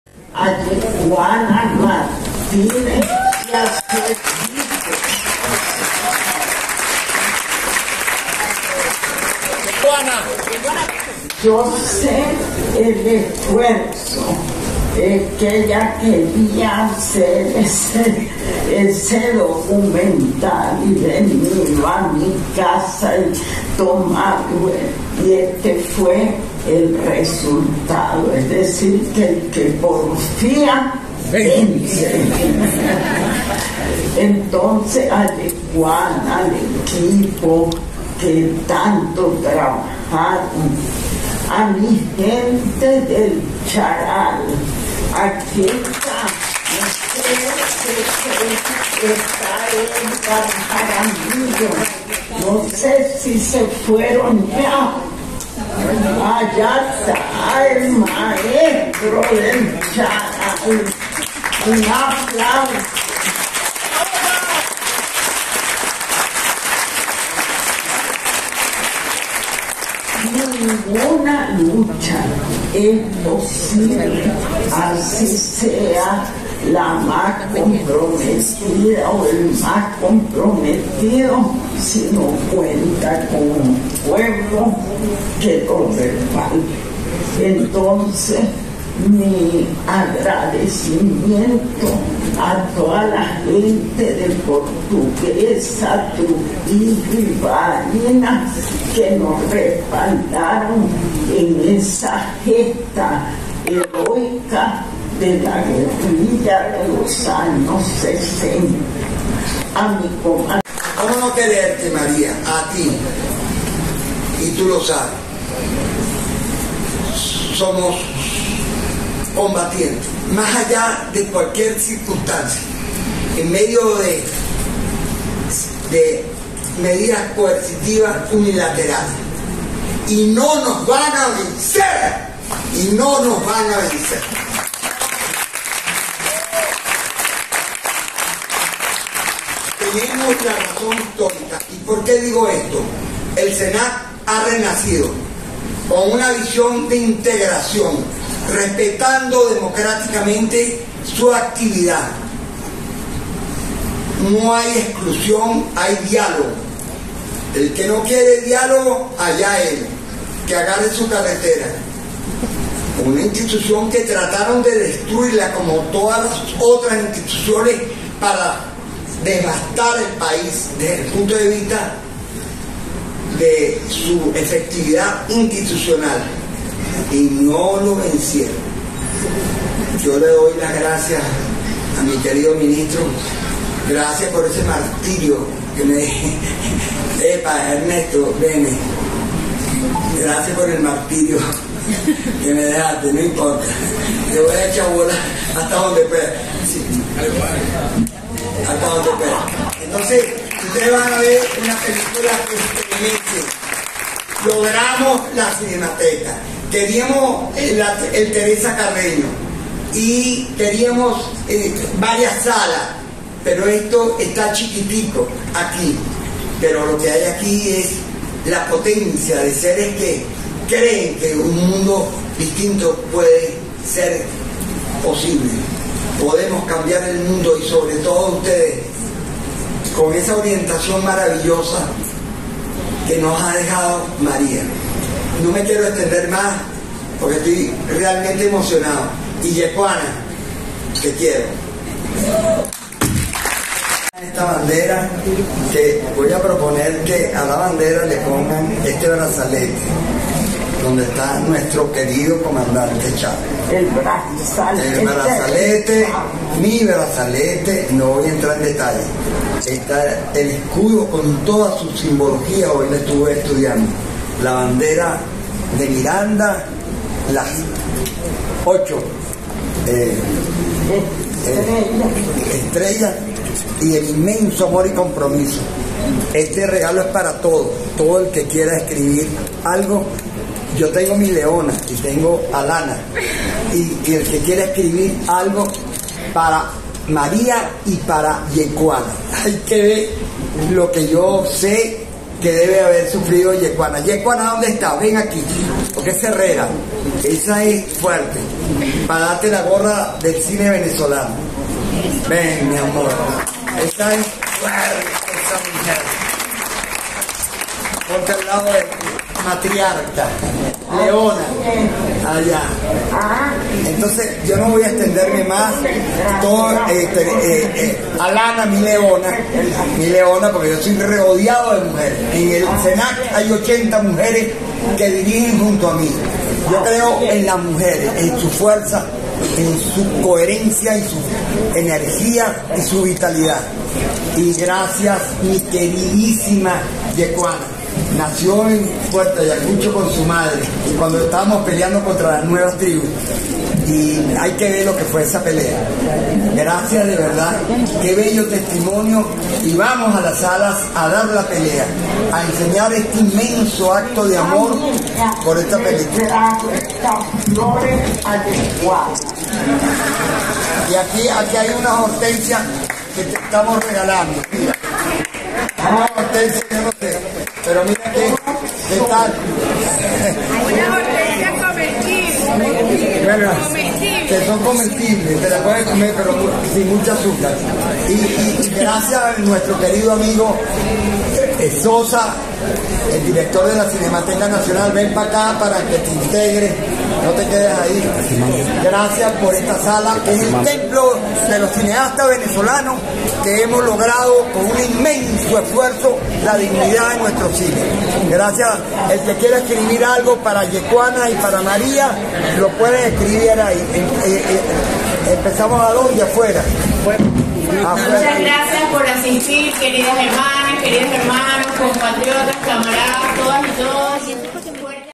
Estrenaron documental "Hortensias para Inés" en homenaje a María León
Videos por Prensa del Ministerio para la Cultura. "Hortensias para Inés" es un documental realizado por Yekuana Martínez, estrenado este 16 de julio de 2023 en el Celarg en Altamira, Caracas. Palabras de María León y de Carlos Azpurua, cineasta y director del Centro Nacional Autónomo de Cinematografía.

USDT o Bitcoin (BEP20): 0xef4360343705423a24119205eeb1de2a10fd23f4

USDT (TRC20): TQJB86JZjF5Kt1C7f322aax4iyD6x65UcV

Petros: PFfDUW3cj1WD7cUPYg9MK2EtgG5nUgVzuc